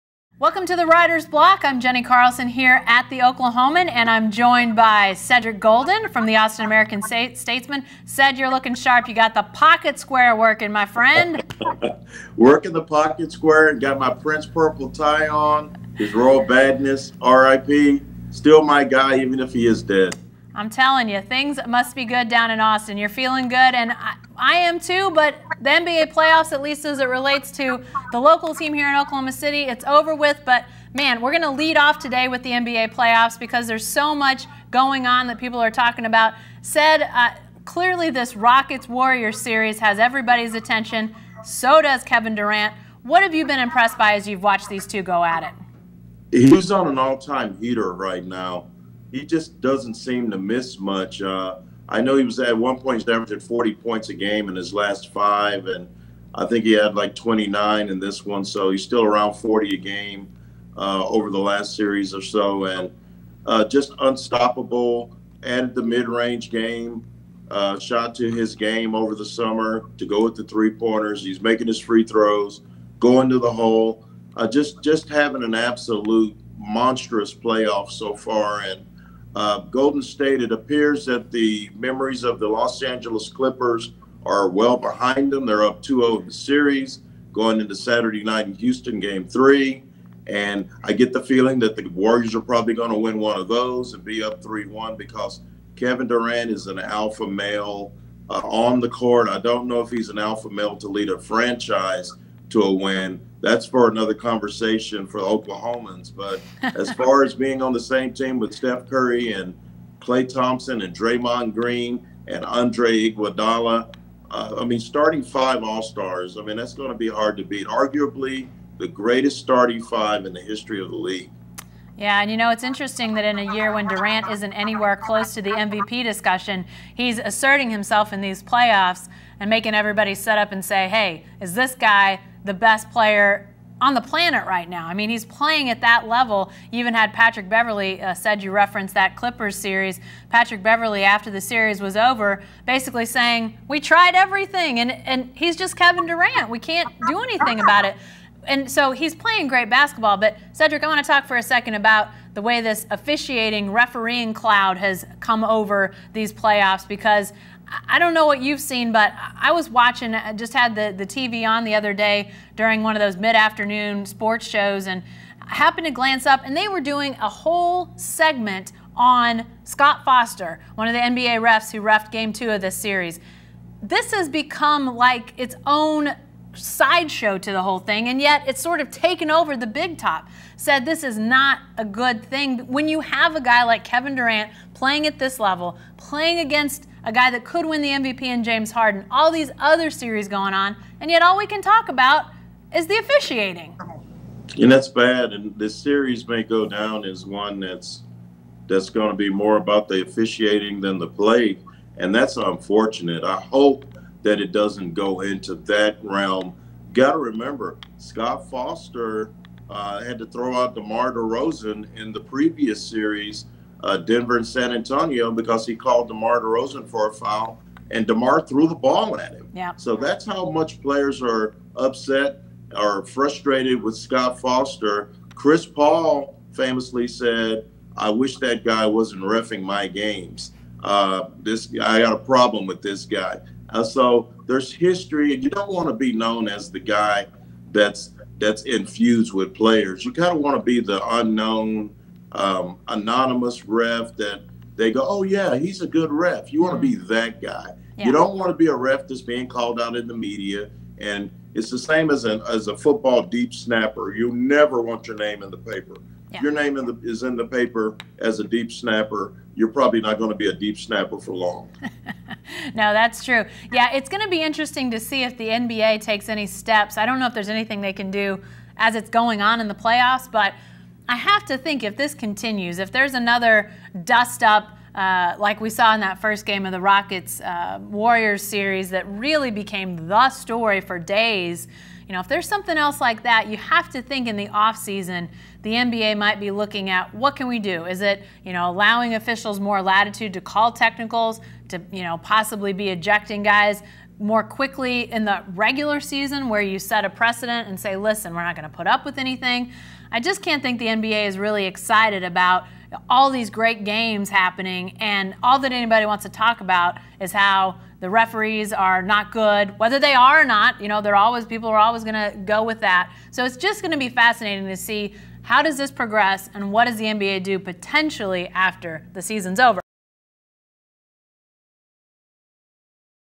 Writer's Block - NBA Playoffs
The NBA Playoffs are in round two and in the Rockets-Warriors series, the referees and Kevin Durant are the topics of conversation.